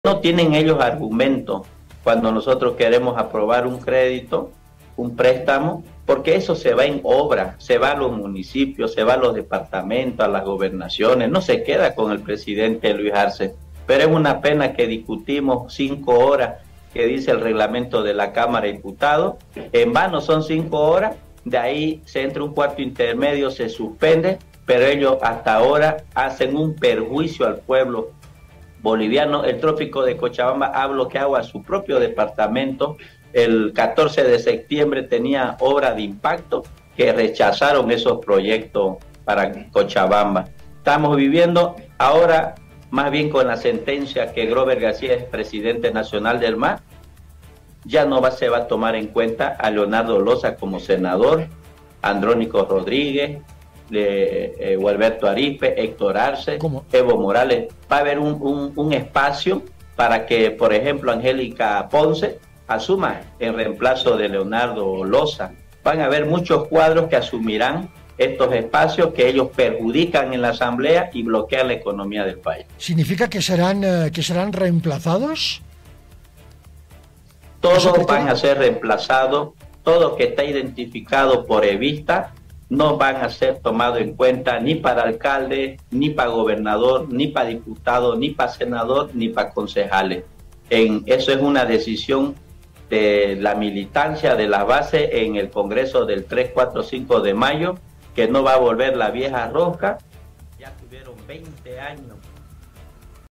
Evistas no serán considerados cuando la nueva directiva del MAS tome las riendas del partido
El diputado Rolando Cuellar anuncia que una vez legitimada en su totalidad la nueva directiva del MAS, senadores y diputados evistas serán alejados de sus cargos en la ALP y no serán considerados para candidatos en las subnacionales próximas. Cuellar mencionó a figuras como LeonardoLoza y AndrónicoRodríguez.